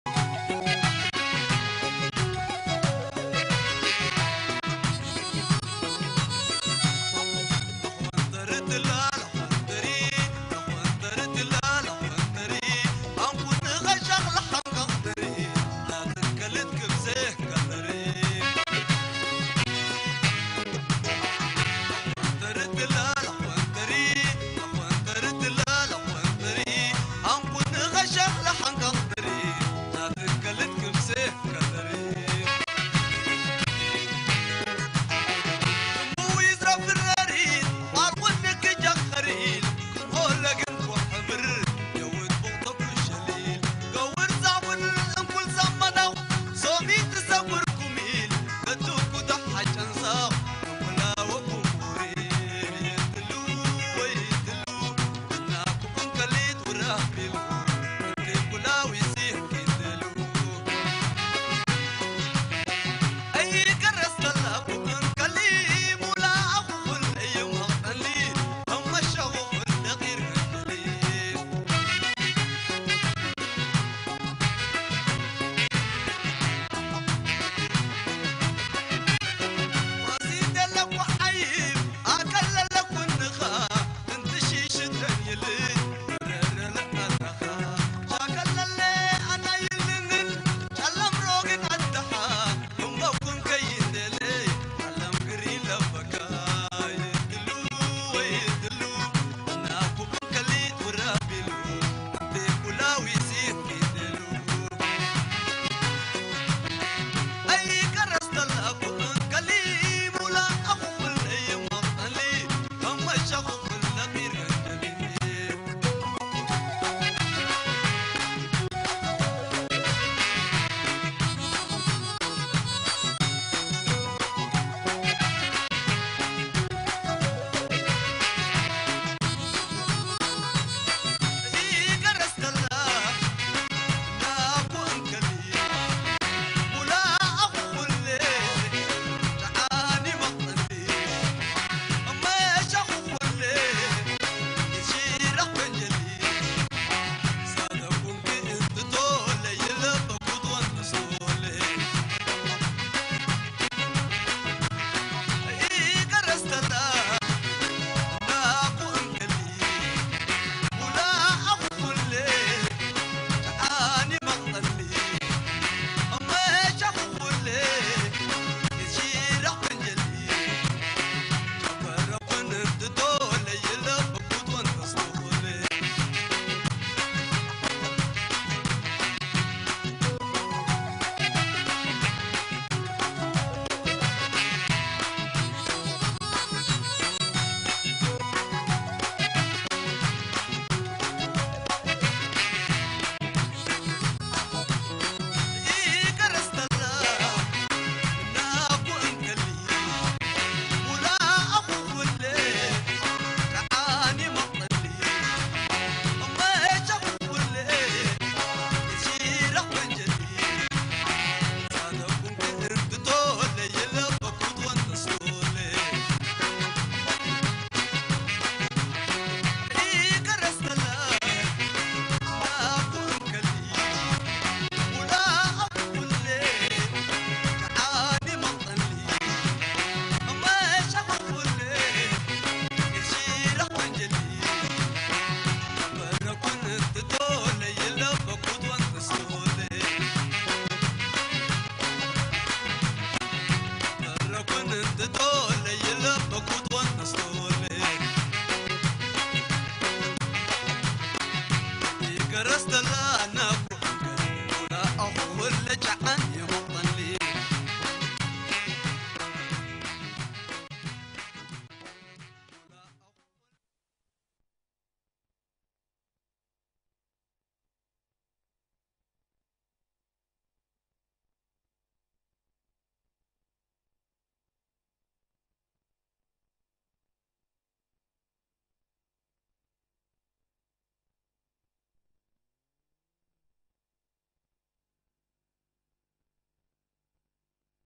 ERITREA : - Bilen Song by "Melekin Atombos"
Bilen Song by "Melekin Atombos" one of the new Talented ERI singers, Most of his songs are in Bilen, Tigre and Tigrinja